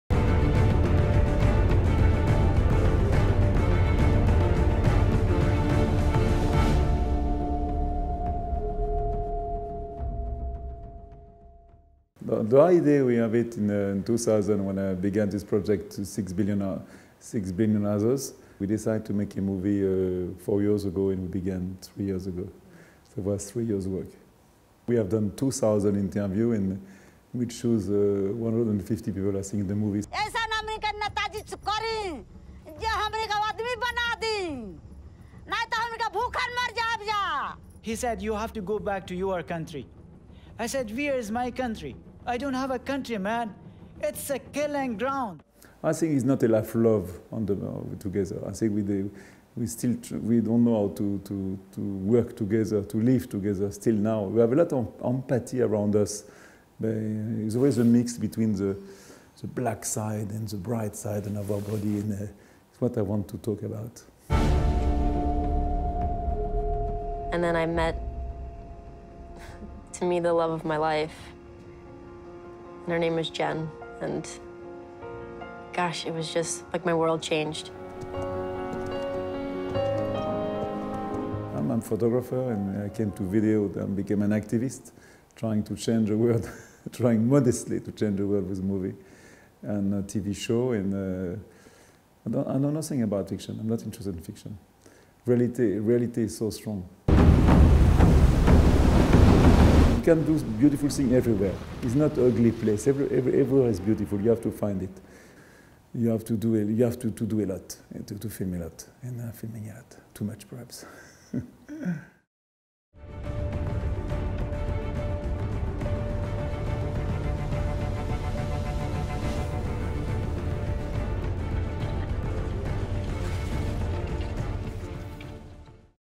Interview with Yann Arthus-Bertrand
Yann Arthus-Bertrand is a French photographer, journalist, reporter and environmentalist. He has starred in the original OSS 117 film (not the one with Jean Dujardin). Arthus-Bertrand started taking photos in the 1980s of the Paris-Dakar rallies, Roland Garros and general wildlife for magazines such as 'Paris Match' and 'GEO'.